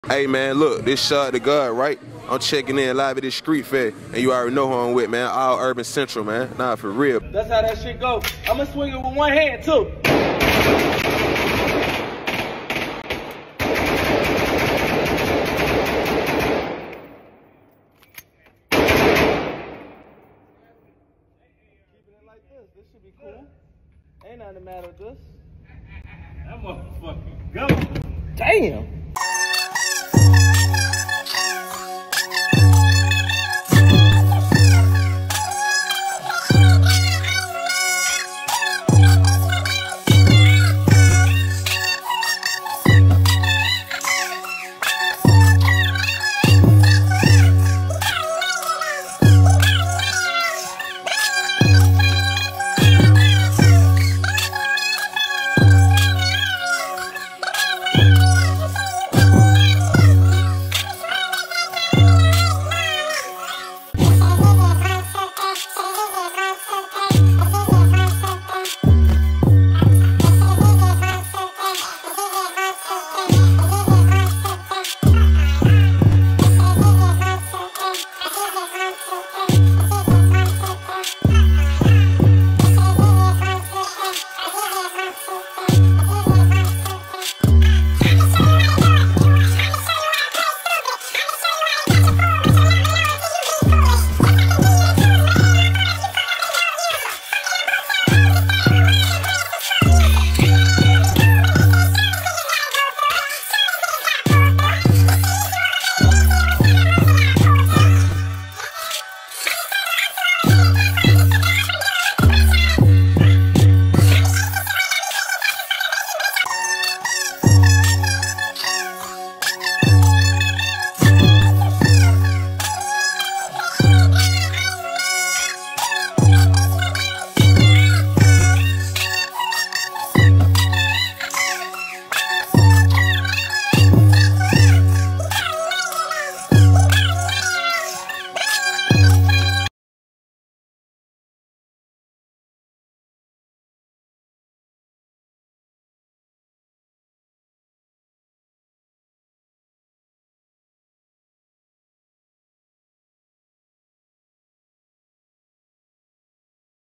Meek Mill Blast Draco Gun One Handed Going Crazy At Rang
Meek Mill At The Gun Range